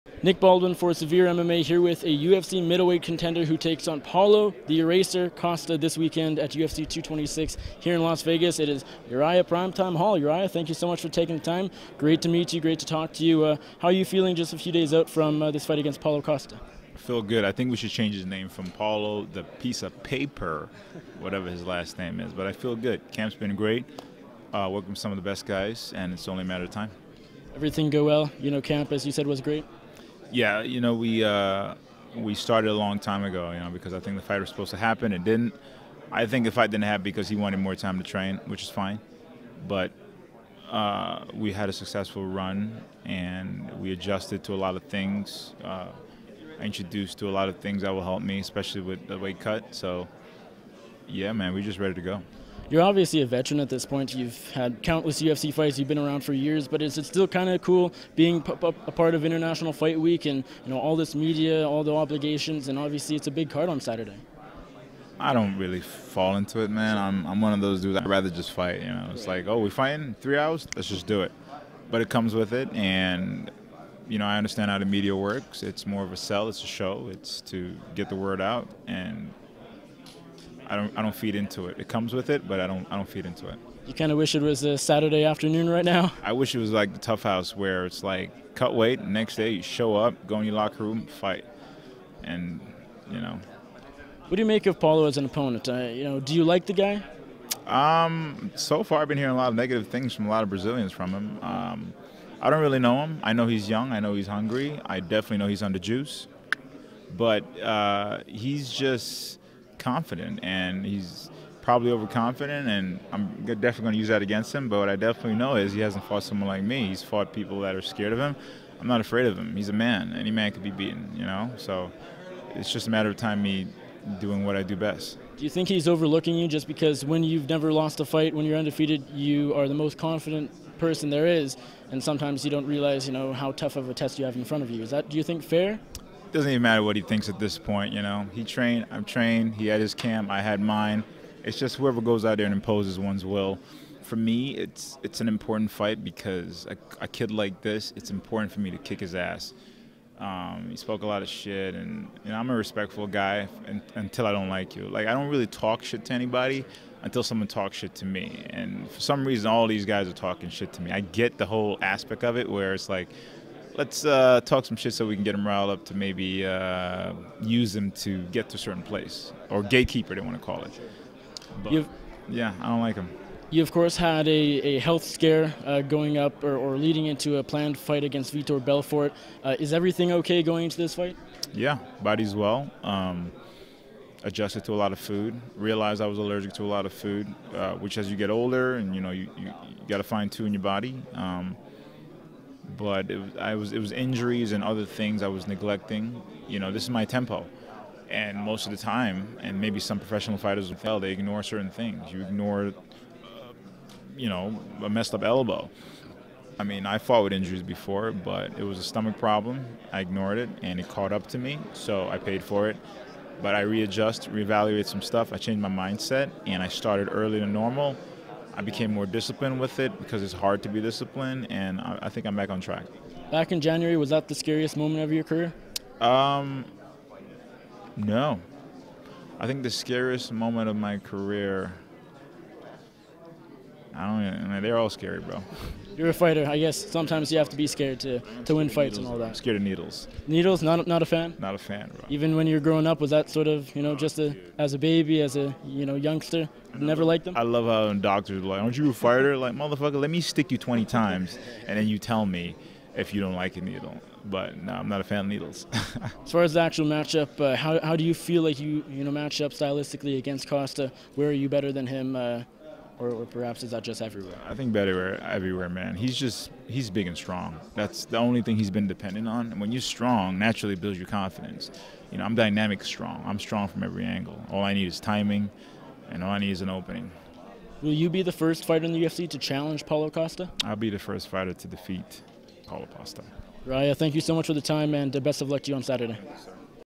Uriah Hall on Paulo Costa: 'I definitely know he's on the juice'
Nick Baldwin speaks with UFC middleweight Uriah Hall at Thursday's UFC 226 media day about his upcoming bout against Paulo Costa on Saturday in Las Vegas, his recent health scare, being afraid of needles, and more.